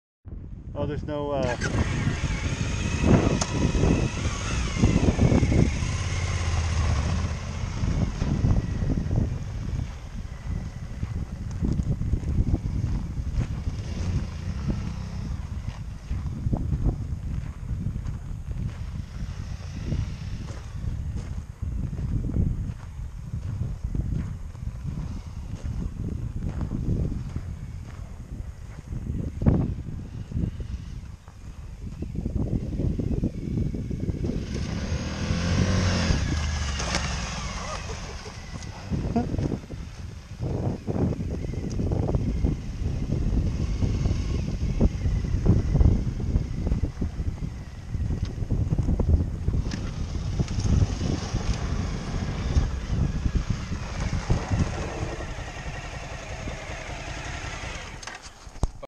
John goofing around in the gravel near Chisasibi - James Bay Quebec
Goofing around on my 2016 Suzuki V-Strom 1000 SE. Gravel Pit near Chisasibi QC